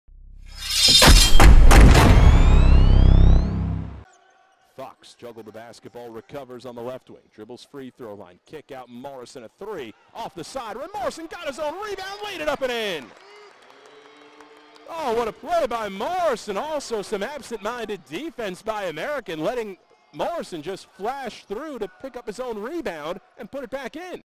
Army Men's Basketball: Morrison Rebound and Layup vs. American 2-11-17
Luke Morrison collects his own rebound and lays it in against American.